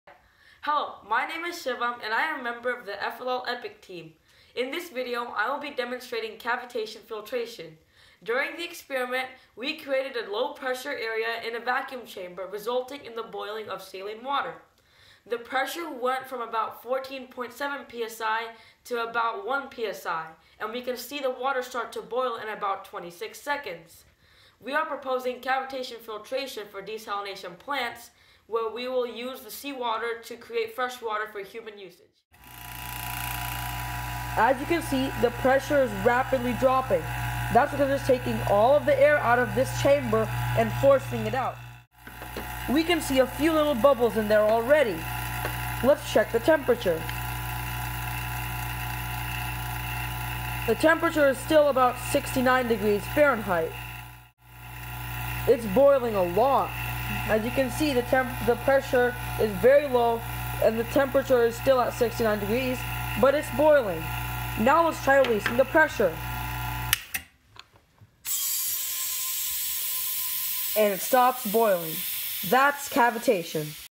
Desalination Via Cavitation
Boiling water by decreasing pressure instead of heating.